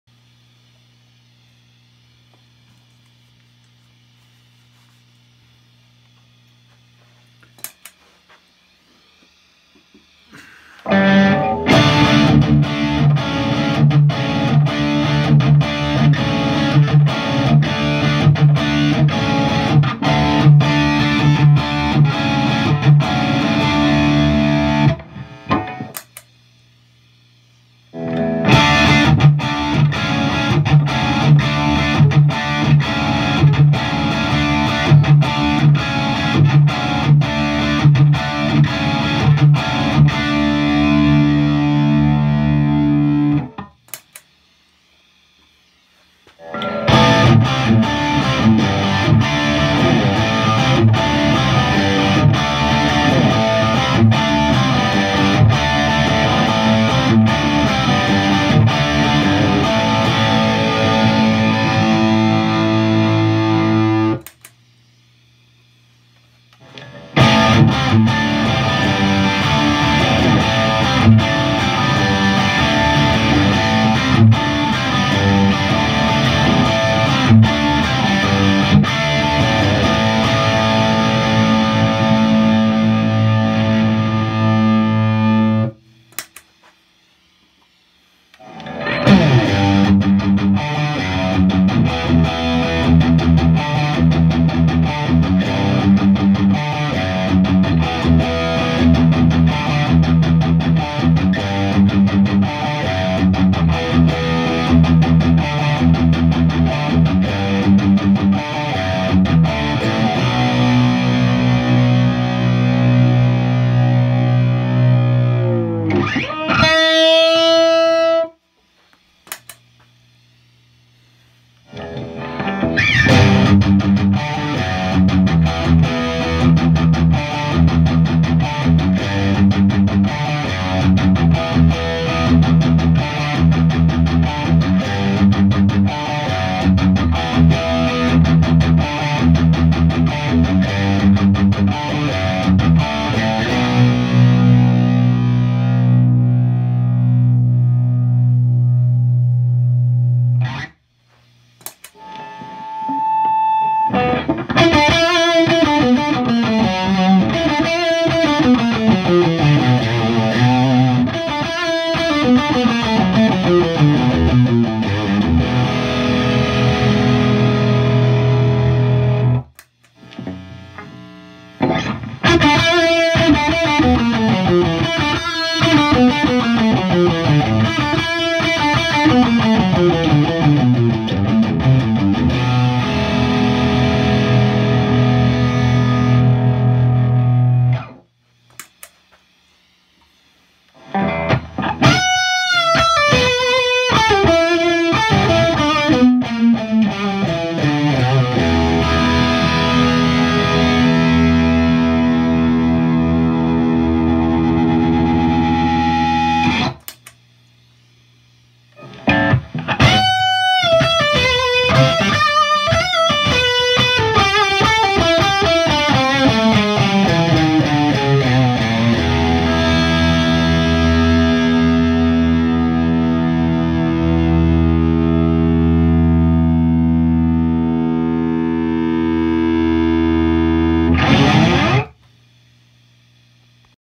Friedman Marsha vs Ss100 Steve Stevens - Into Marshall 1977 with Blackbacks
Friedman Steve Stevens vs Friedman Marsha 100 watt. The Marsha was set to sound close to the ss100. Settings are close but the Marsha has a thump knob on the back that is turned to 3 o’clock. C45 is on. No other switches on. Fat did not match the correct frequencies.

Played through a marshall 1977 slant cab with blackbacks.

Radial headbone amp switcher.

Kramer night swan with a full shred tuned to e.